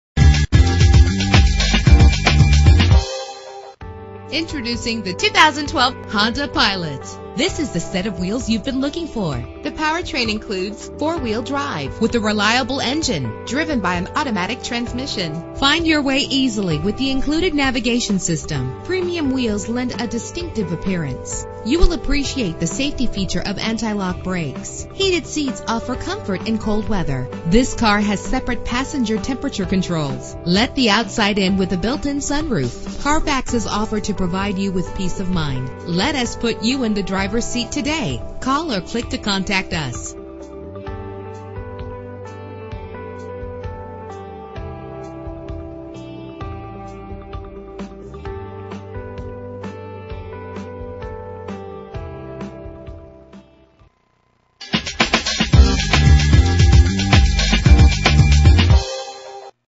2012 Honda Pilot - Amherst OH
Year: 2012
Make: Honda
Model: Pilot
Trim: EXL
Engine: 3.5 L, 6 cylinder
Transmission:
Color: Black
Mileage: 70411
Address:
47190 Cooper Foster Park Rd.